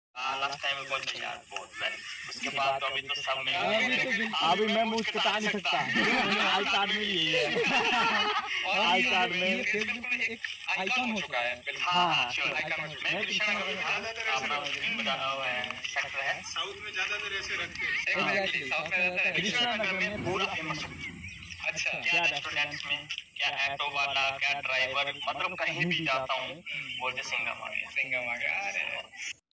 BSF jawan viral video जियो और जीने दो की सच्चाई fake video please don't🤬share जवान अभी तक जिंदा है
BSF